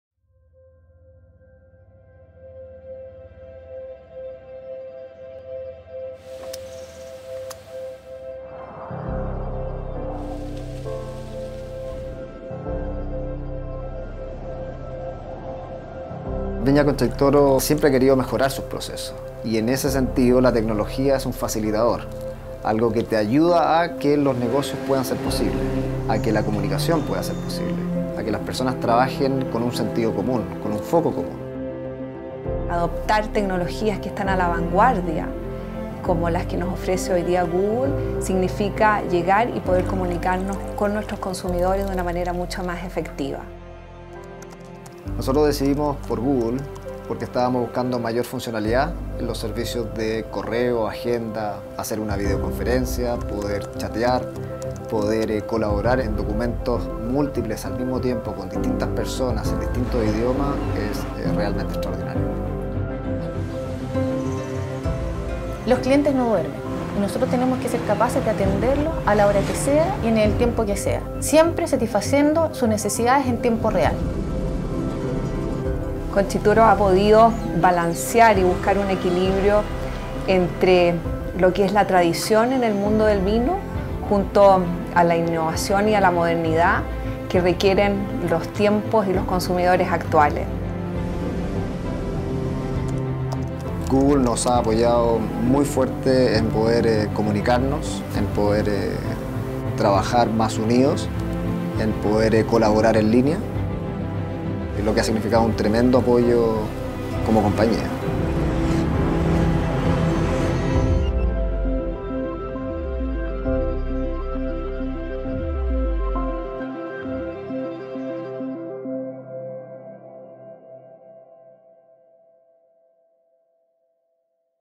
Google Apps for work | Concha y Toro
Corporate video for Google and Concha y Toro about google apps for work.